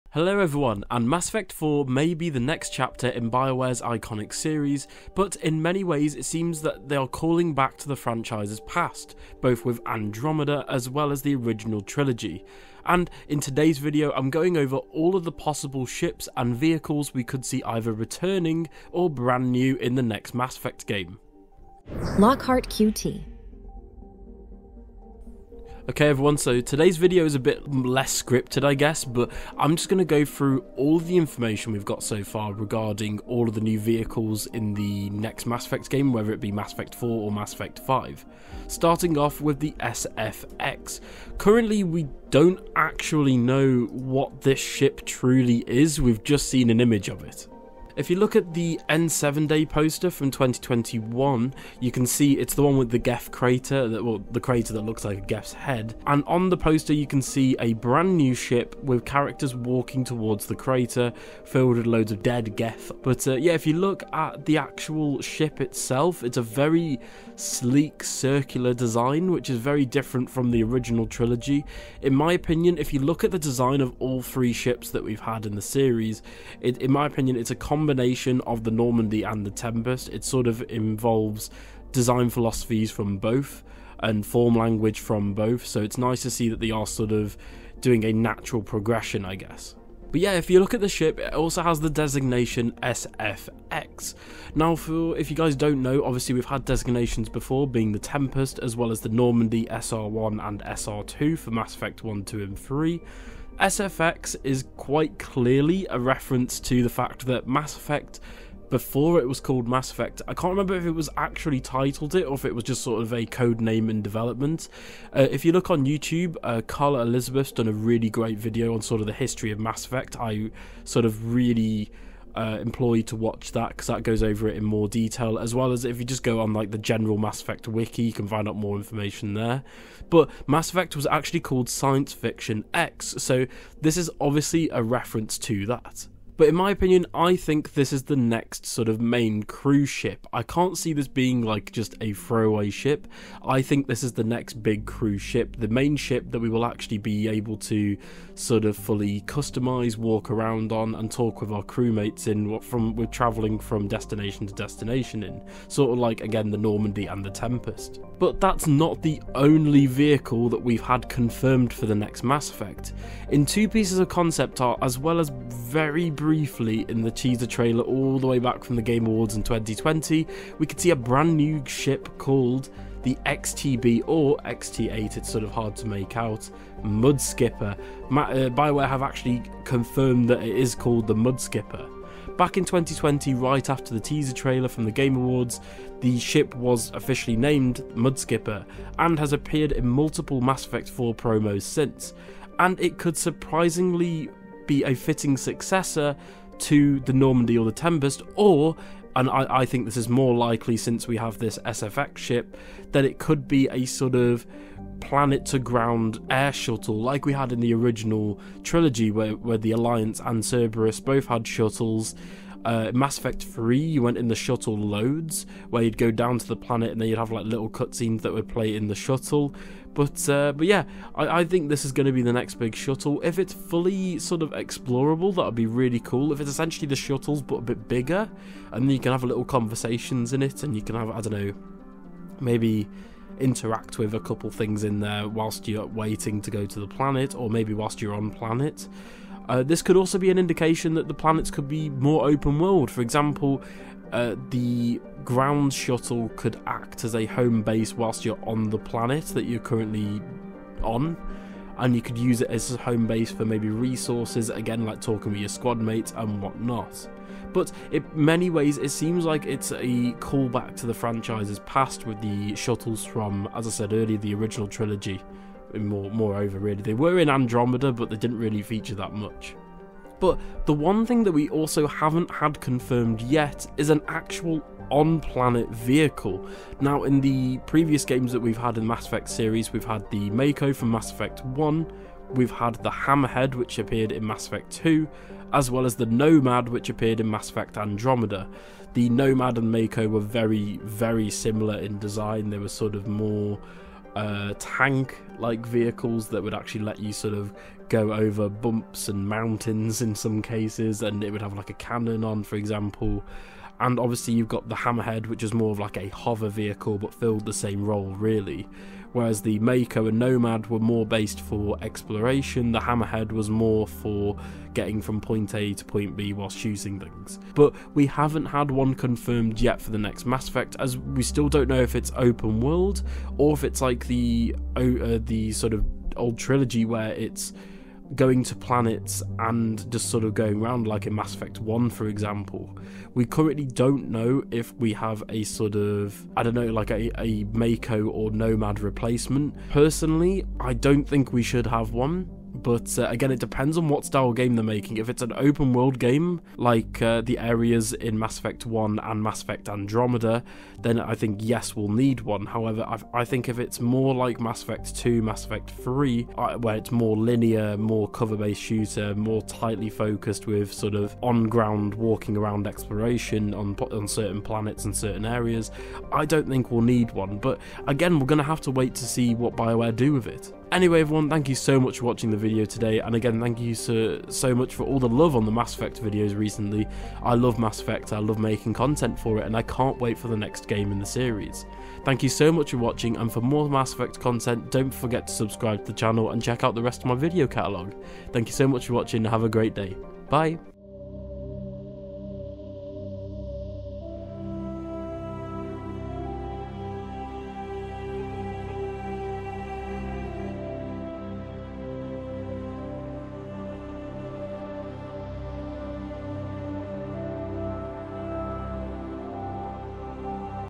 MASS EFFECT 4 - NEW SHIPS & RETURNING VEHICLES!?
This video is a quick deep dive into all of the confirmed and speculated ships & vehilces coming in the next mass effect game in the series.

___TIME STAMPS___
Introduction – 0:00
SFX New Crew Ship -- 0:25
Mudskipper XT8 -- 2:38
Old ships returning? -- 4:30
Outro -- 6:20